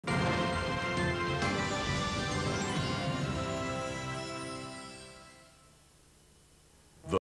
TVW-7 Perth's Home Station ID (June 1998)
Localised ID of Seven's Every One's Home image.